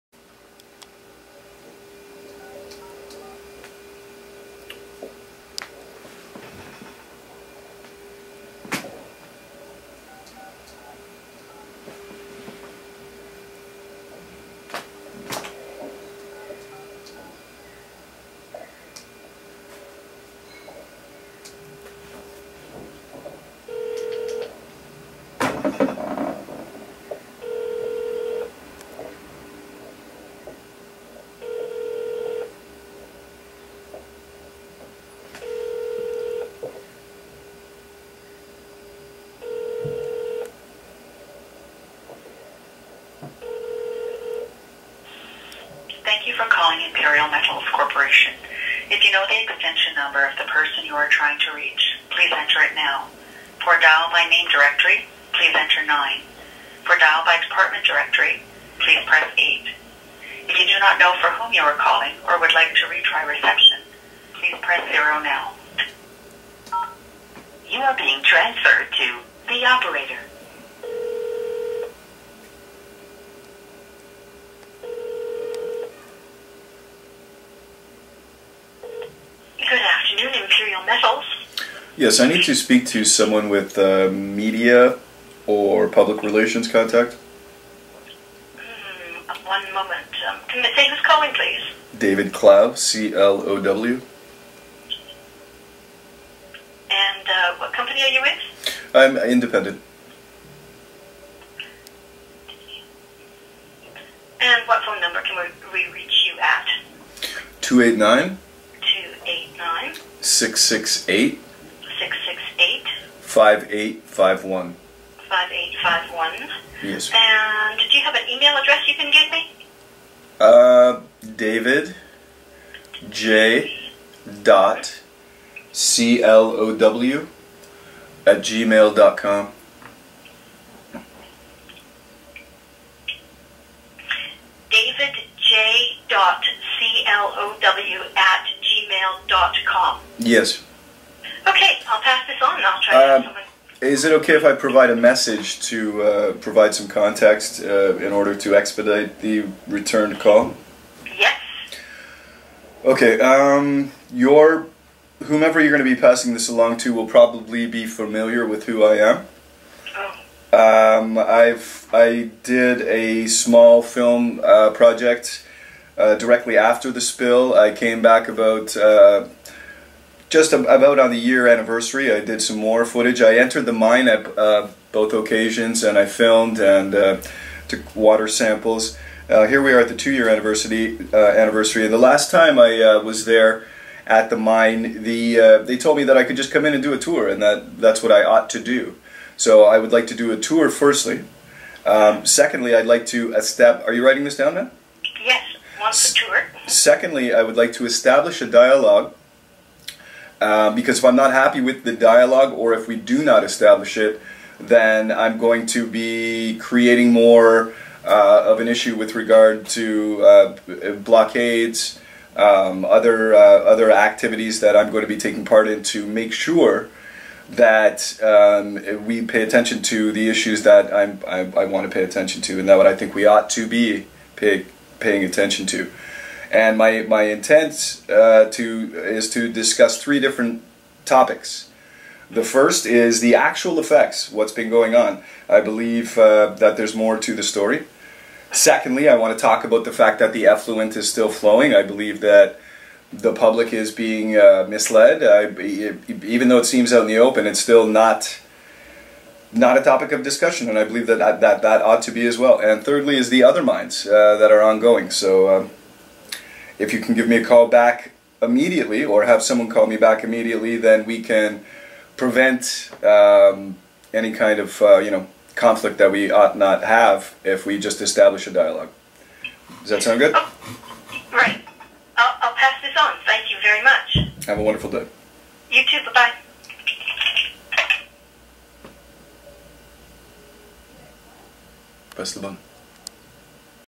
Operation: IMPERIAL POISONS, starring N. Murray Edwards
In the two years investigating and educating myself around the issues of the Mount Polley tailings pond rupture, and the continued flow still flowing two years later, including many visits to 'ground zero' of the destruction, the research shows that this mine, and the ones like it, need to be forced to cease all operations and compensate the citizens of the communities affected and the provinces of Alberta and British Columbia, and the state of Alaska, for the wanton negligence that resulted in the largest environmental disaster in Canadian history. This is the beginning of a series of actions I will implement to raise awareness to the current science that I will put to those still perpetrating this ongoing disaster - N. Murray Edwards, Imperial Metals, and its subsidiary in the Mount Polley Mine. This is the beginning of my solicitation seeking out an as-yet unestablished dialogue. There has been no punitive consequence on the perpetrators of this destruction. There has been no solution nor resolution; nor has their been any compensation. But there has been much profit to this very shameless man in N. Murray Edwards, the owner of the Calgary Flames and the profiteer of ecocide. Anyway, I have done my homework; and it doesn't look good for Imperial shares.